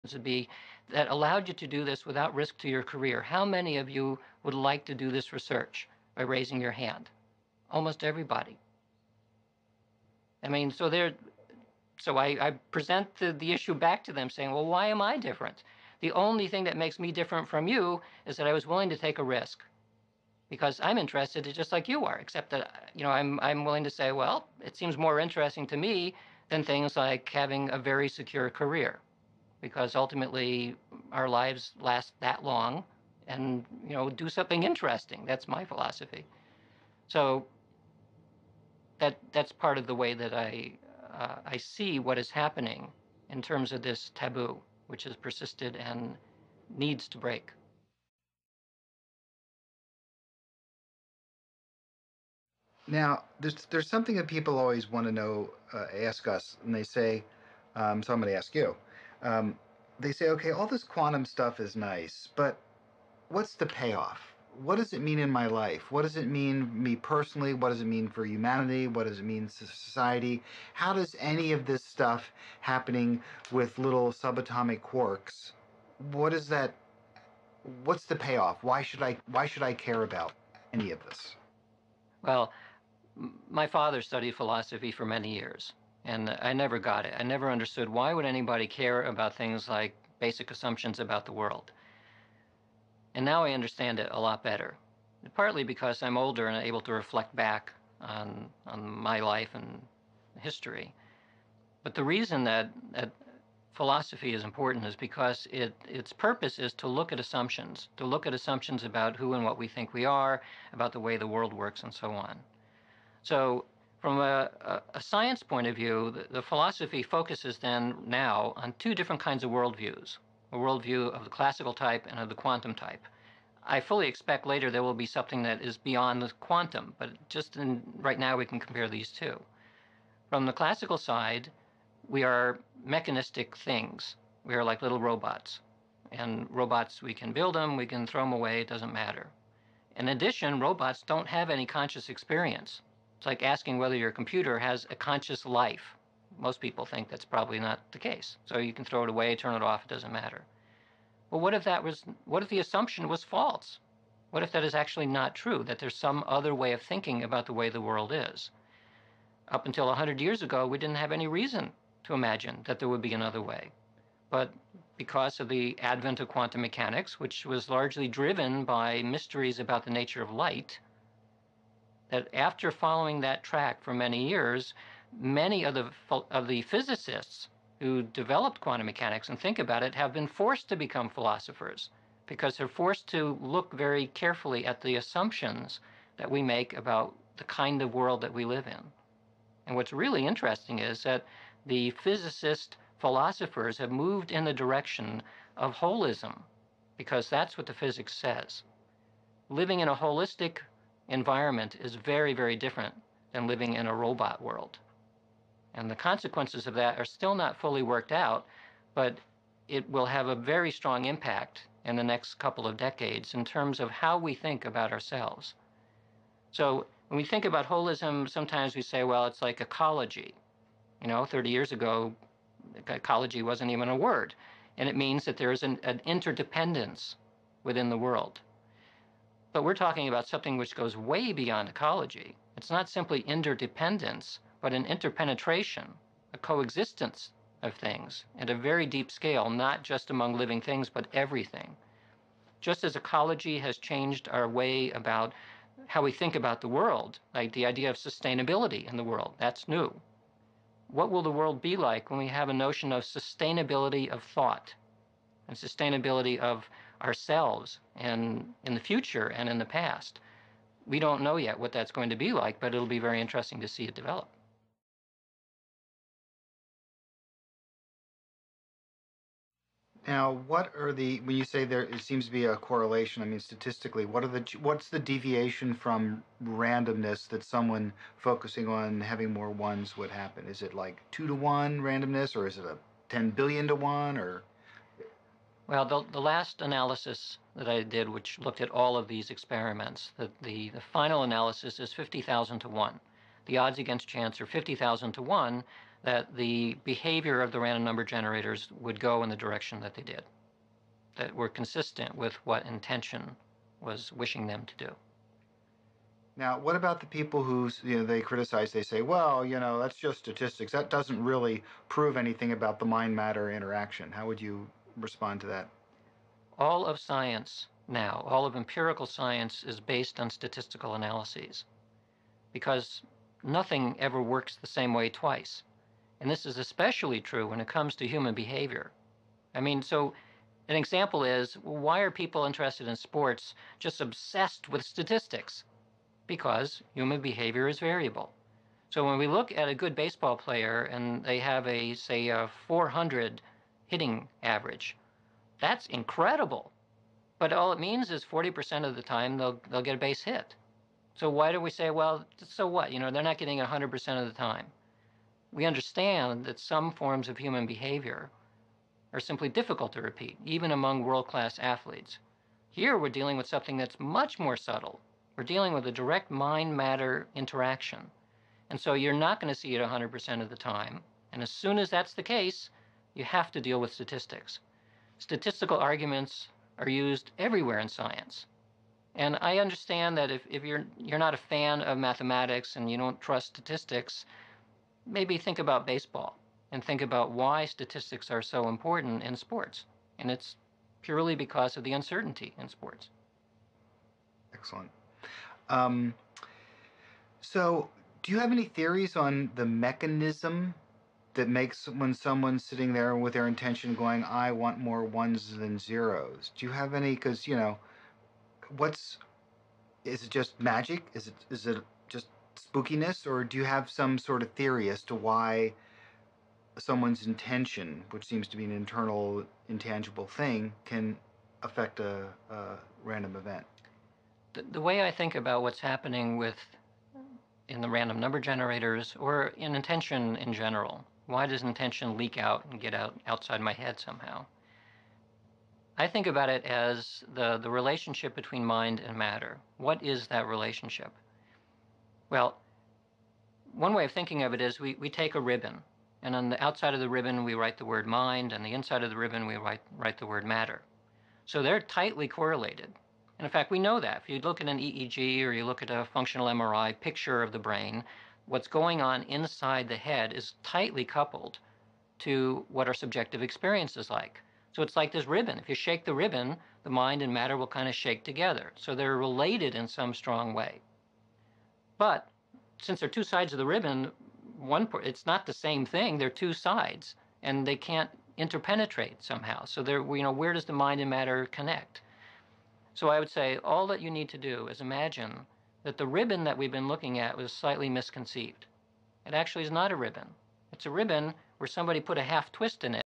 Dean Radin, Ph.D on Quantum Physics 2 of 3
Dean Radin, Ph.D on The Stupidity Hypothesis, Quantum Physics and Mind Matter interface